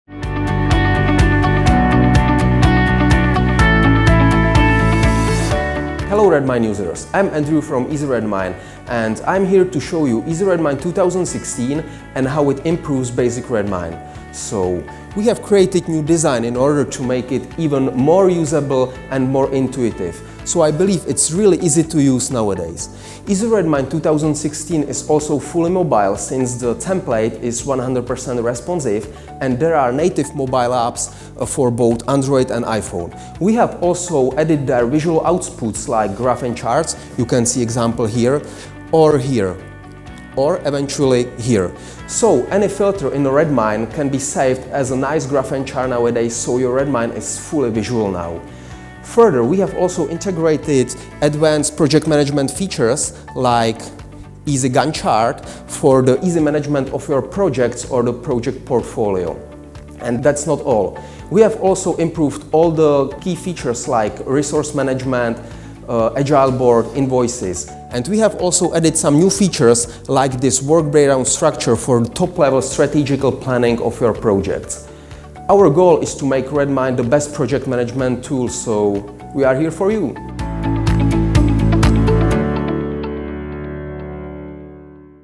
What is new in Easy Redmine 2016? And how it improves basic Redmine?
Easy Redmine 2016 is here - check out 1-minute video about new and improved features that can be a big help for you project management!